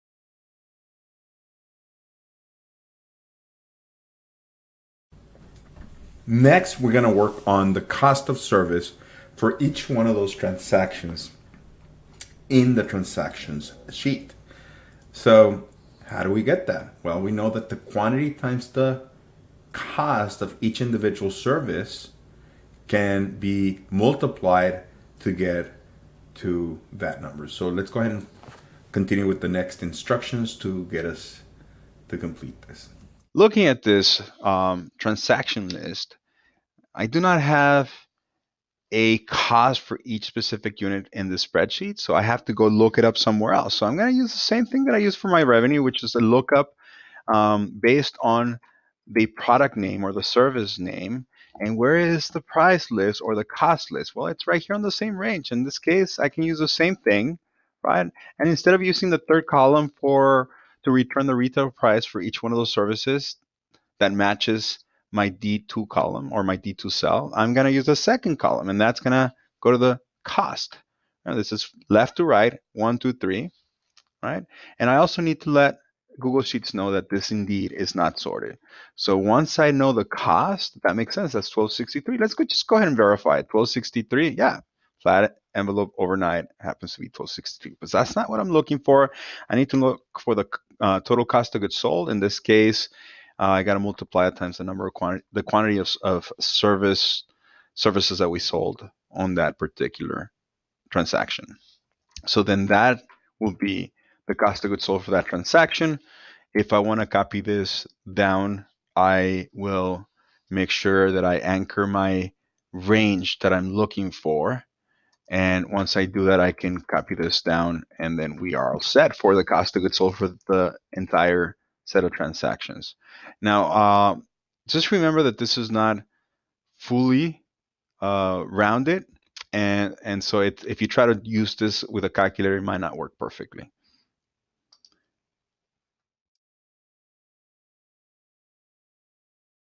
Shippy Express Step 3 - III
Shippy Express: Augmenting accounting education with Google Sheets

Authors:
Fernando Parra, PhD CISA
Aimee Jacobs, PhD
Laura L. Trevino, PhD

Creative Video Design:
Giovanni Parra

Abstract: The purpose of this teaching case is to provide you with an exercise that uses cloud-based Google Sheets as an alternative to Microsoft Excel to learn spreadsheet functions from a managerial accounting perspective. Specifically, you will use fictitious data from Shippy Express, a shipping company seeking advice from your accounting firm, to develop managerial summaries of their financial transactions to help them make training decisions. You will be supported with instructional YouTube videos intended to guide you at any level of expertise. The case is particularly useful for asynchronous learning as it provides you and your instructors with real-time access to your work-in-progress, versioning audit trails, and a dynamic set of feedback functionalities that augment student-instructor collaboration. It will operate well when integrated into an accounting information systems or managerial accounting course.

Shippy Express, an express shipping service provider, wants to analyze the revenue and profit margin of a subset of transactions belonging to one of its Sales Representatives, Veronica, to better understand her contribution to the company. The shipping service company has contracted the services of Super Managerial Accountants (SMA), where brilliant students are doing internships, to help with the analysis. The information from Shippy Express, however, was extracted in a format that requires the connection of relational data contained in separate sheets. Shippy Express would like for SMA to generate a descriptive visualization and data summaries of the sales based on customers, industry, and regions. SMA is also tasked with analyzing each transaction’s operating margin and comparing it to its industry average. Shippy Express wants to be able to access SMA’s work at any time and has requested that SMA use a cloud-based solution to collaborate in real-time using spreadsheet functions to summarize this information. Student interns at SMA will use Google Sheets to allow the supervisor at SMA (instructor) to oversee the work and offer guidance if necessary.
The first sheet of the Shippy Express workbook contains general information, including the list of Shippy Express’s customers, the pricing and cost list for shipping services, and a data dictionary table with abbreviations used for classifying sales regions. You will use cell referencing from the General Information sheet to fill in the missing data (e.g., industry for a customer, cost of service) on the Transactions sheet.